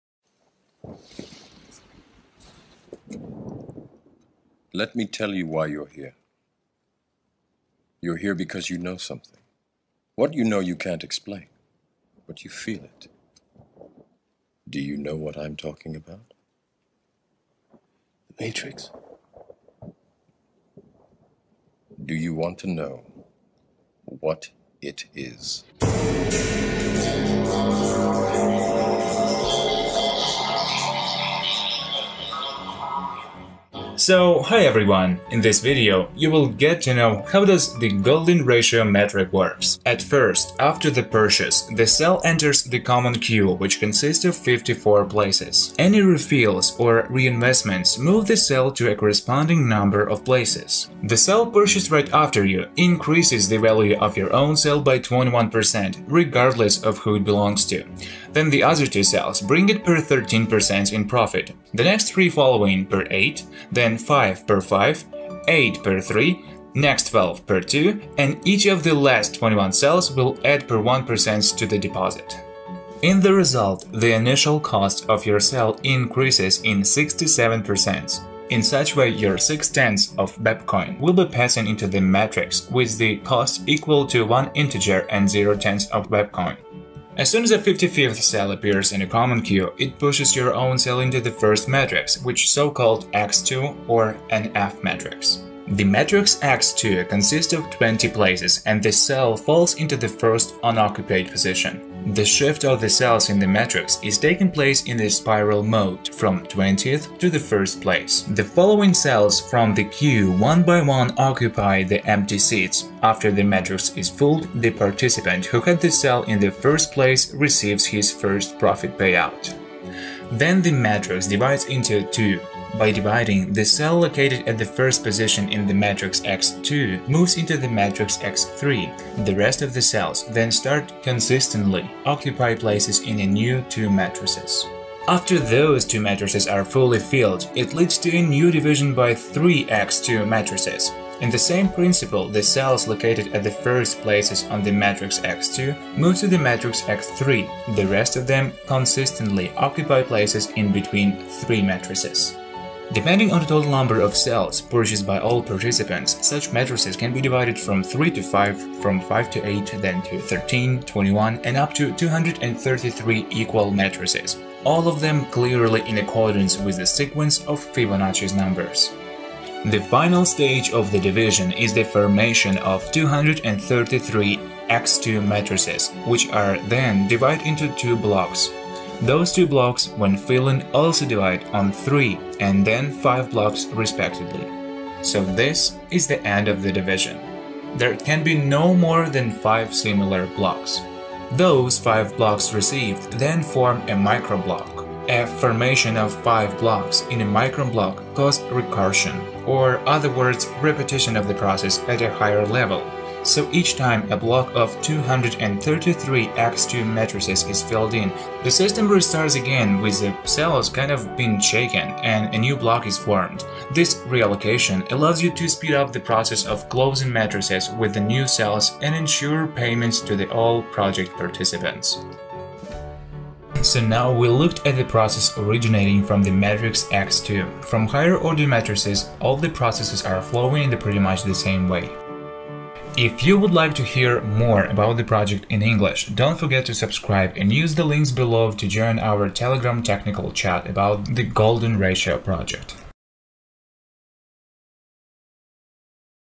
HOW GOLDEN RATIO MATRIX WORKS
Non working income
link

Do you want to get 11936 WEC coins from 30 WEC coins ?

Participate in our new project, register using the reflink below-in the signature))

"WHERE DO SUCH LARGE PERCENTAGES COME FROM?", READ HERE -  To view links, please register.

Only 5 levels of payouts.

After completing all 5 matrices, you will earn 39787%.

As you progress, there will be 5 payouts in the sequence:

20 WEC + 72WEC + 420 WEC + 2688 WTP + 8736 WEC = 11936 WEC

Investments in spiral marketing allow you to receive the first interest accruals automatically after passing the General queue.

The participant receives an absolutely predictable fixed profit of 167% in just a few seconds of working in the system, and then gets into the matrix.

After passing the General queue, You have already received the first profit-you invested 30 WEC in the purchase of one cell, and received 50 WEC.

Now you have the opportunity to go to the main part of the marketing plan "Fibonacci Matrix — - a spiral marketing system that has no analogues among other investment projects.

The cost of one cell here is 50 WEC tokens. Immediately after paying for it, You get into the f-matrix, consisting of 20 cells, one of which belongs to you. The total profitability of the project is almost 40,000% — if you invest 30 tokens without any additional effort, you can get 11936 WEC.

And this is only from one cell, the number of which for each participant can be any.

*One of the main features of the project is that it is not necessary to invite partners to your team. A shared queue is active. You can pass all the matrixes without a single referral!*

The project has just started! On November 21! You have a great opportunity to go at the start. And watch as a huge team builds up behind you!

For active participants - referral Commission of 5%

it is a long-time investment. But you will be happy when you will see that your deposit gave 40 000%!!!

Everyone can pay for an unlimited number of cells at a set price (from 0.06 to 0.6 WEC, depending on the conditions in the Guild). After the purchase, the cell takes a place in the common queue. In this case, the cell acquires its own unique and unchangeable number.

The next cell purchased after it increases the value of the first one by 21%, regardless of who it belongs to. The next two cells will bring her 13% profit, the next three — 8%, the next five — 5%, the next eight – 3%, the next thirteen — 2%, and another twenty-one cells will add 1% to the Deposit. In other words, the Fibonacci numerical sequence allowed us to calculate the optimal percentage of profit for each participant. Let's look at marketing using the example of the Fibonacci Guild.

There are 54 places in the common queue. With a large number of cell purchases, the queue moves very quickly, and the cost of a cell increases by 67% in a few minutes. That is, after passing the common queue, the initial cost of one cell from 0.6 WEC (in the Fibonacci Guild, in other guilds on a different plan) increases to 1 WEC.

Exchange link to buy WEC (web coin pay)